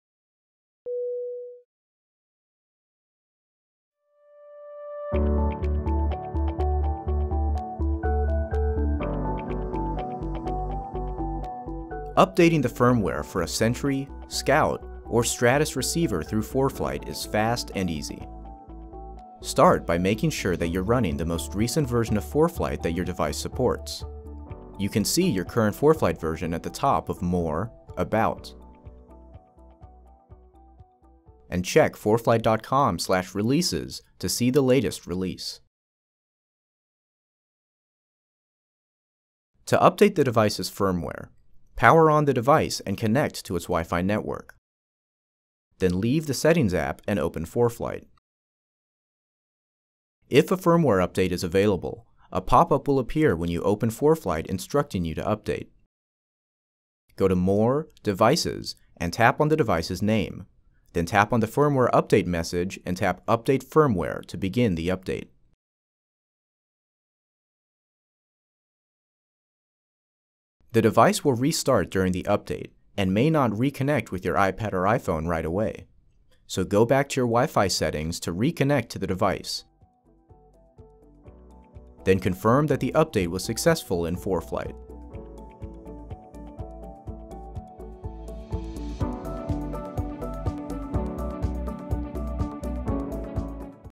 ForeFlight Quick Tip: Update Firmware for Scout, Sentry, & Sentry Mini (see description)
*NOTE: Stratus is no longer updated through ForeFlight but via the Stratus Horizon App. Scout, Sentry, and Sentry Mini are still updated as shown in the video. Also, firmware updates are no longer tied to ForeFlight Mobile app updates and may become available for your device at any time.*

Updating the firmware for a Scout, Sentry, or Sentry Mini receiver through ForeFlight is fast and easy. Start by making sure that you’re running the most recent version of ForeFlight that your device supports. You can see your current ForeFlight version at the top of More - About, and check to see the latest release.